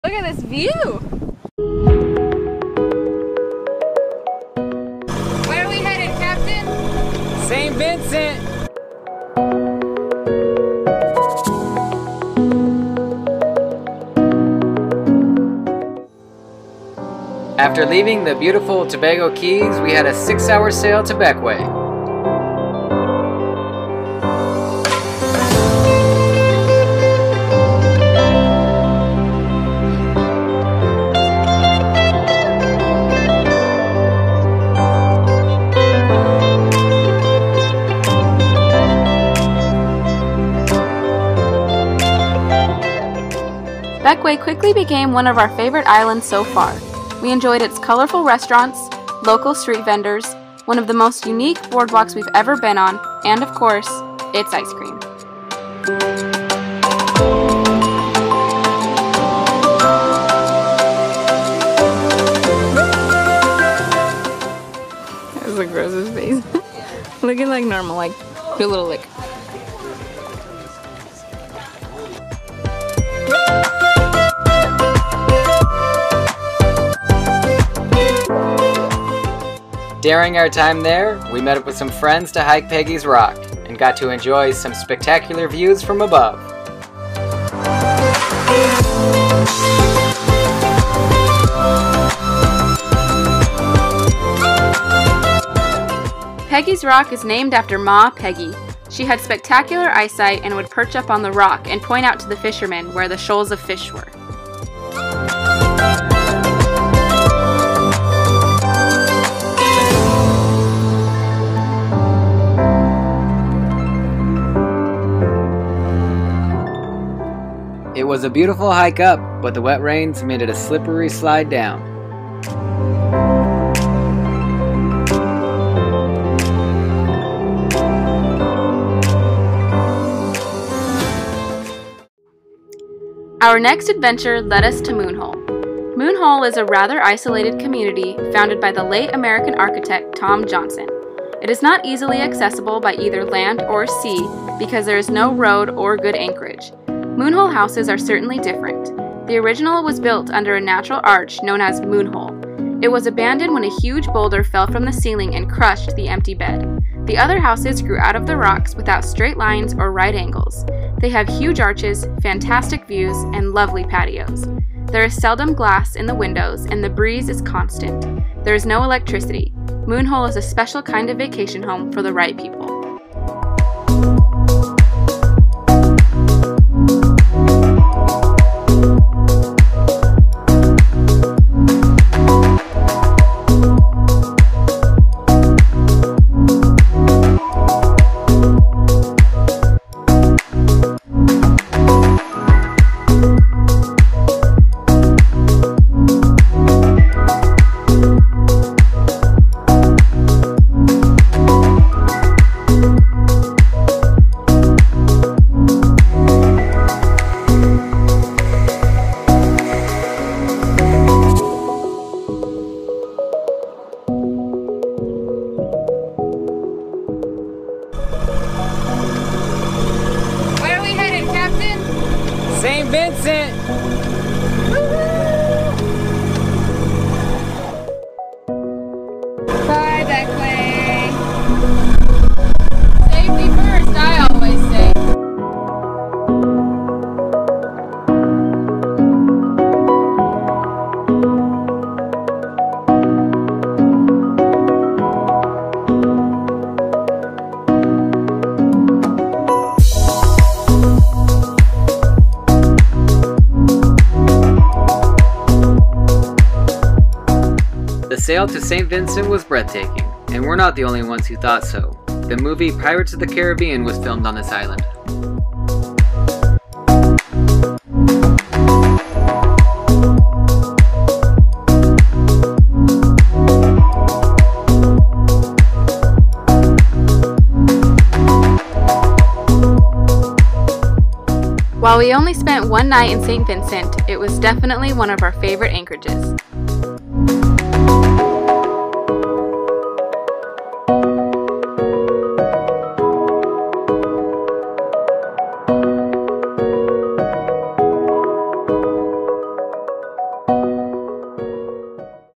Bequia: (Beck-WAYYY Freaking Cool) Ep. 7
We spent this week hiking Peggy's Rock with friends Vela Boat and Sea Yawl in Bequia, exploring Moonhole, and anchoring where Pirates of the Caribbean was filmed. St. Vincent and the Grenadines was beautiful on all accounts and we enjoyed our time there.